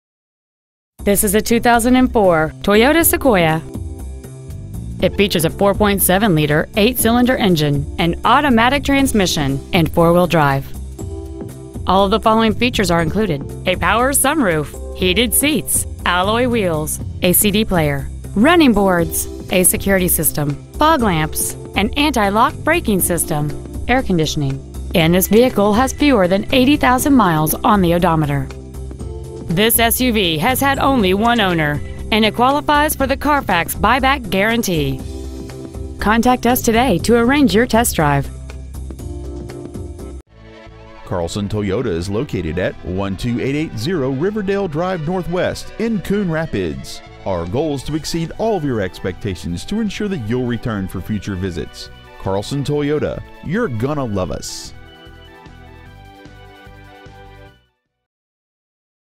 Pre-Owned 2004 Toyota Sequoia Coon Rapids MN 55448
Year : 2004
 Make : Toyota
 Model : Sequoia
 Engine : 4.7L 8 Cyl. Fuel Injected
 Trans . : Automatic
 Exterior : Phantom Gray Pearl
 Miles : 79,254

 Stock : 3186L